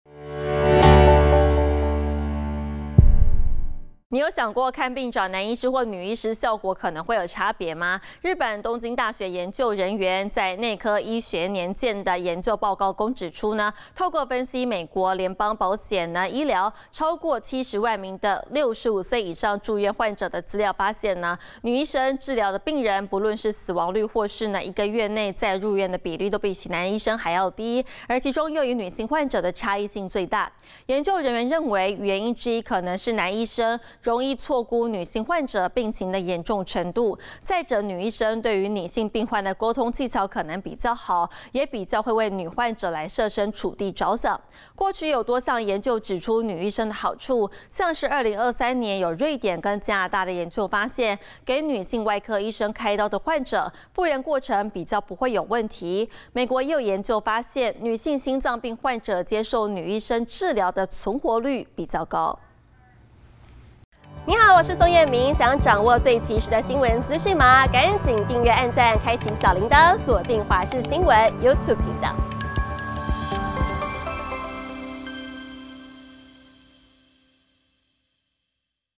女患者找女醫生效果更佳？ 研究「死亡率.再入院率較低」｜華視新聞 20240424
您有想過看病找男醫師或女醫師，效果可能會有差別嗎？日本東京大學研究人員，在「內科醫學年鑑」的研究報告中指出，透過分析美國聯邦醫療保險，超過70萬名65歲以上的住院患者資料發現，女醫生治療的病人，不論是「死亡率」或是「一個月內的再入院率」，都比男醫生低，其中又以女性患者的差異最大。研究人員認為，原因之一可能是男醫生容易錯估女性患者病情的嚴重程度，再者女醫生對於女患者的溝通技巧可能比較好，也比較會為女患者設身處地著想。